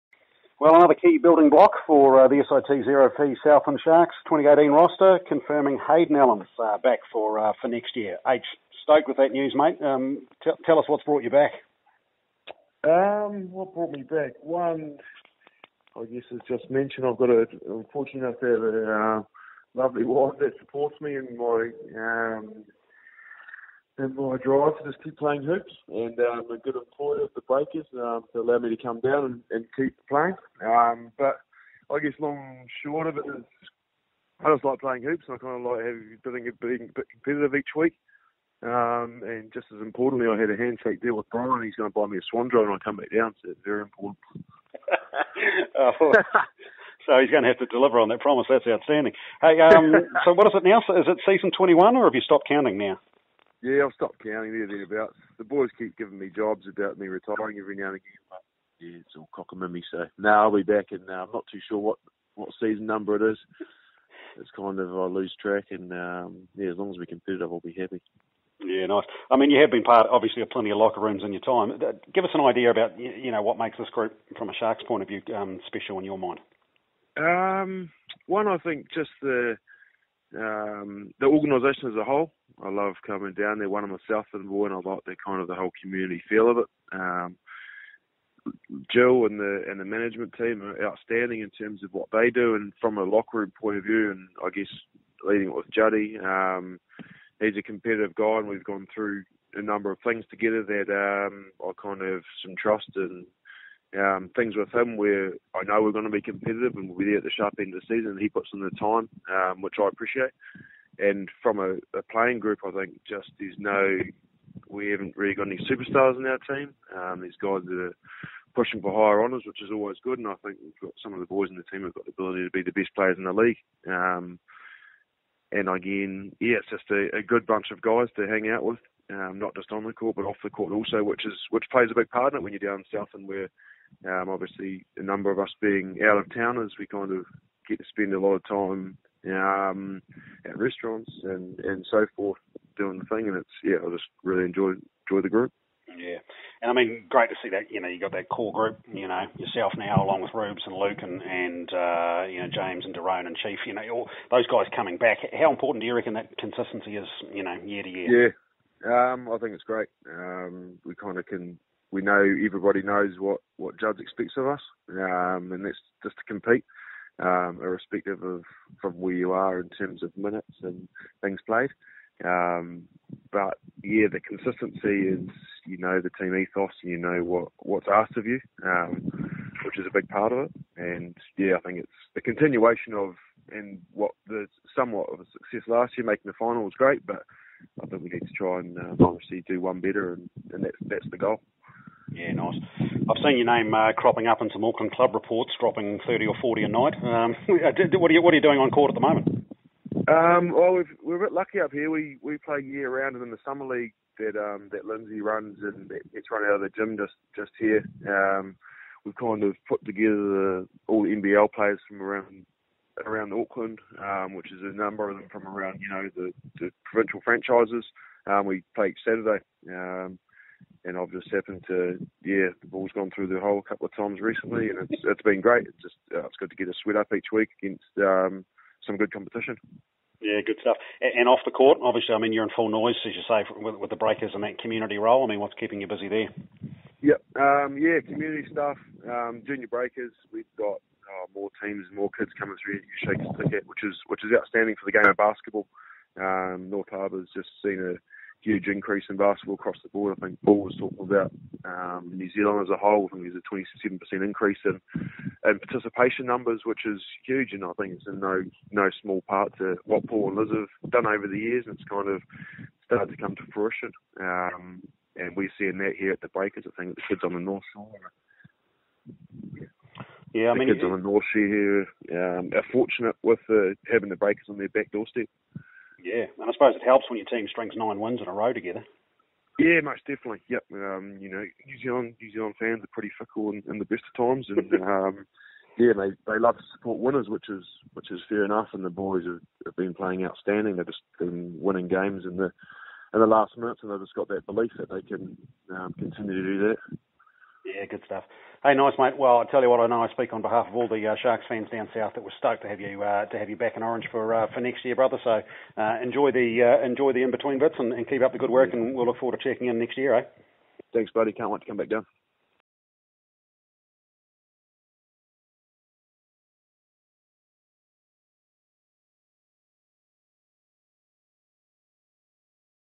Hayden Allen returns for the Sharks
Hayden Allen is back for 2018. The NBL legend chats with Nick Jeffrey about his return to the SIT Zero Fees Southland Sharks for the 2018 New Zealand NBL.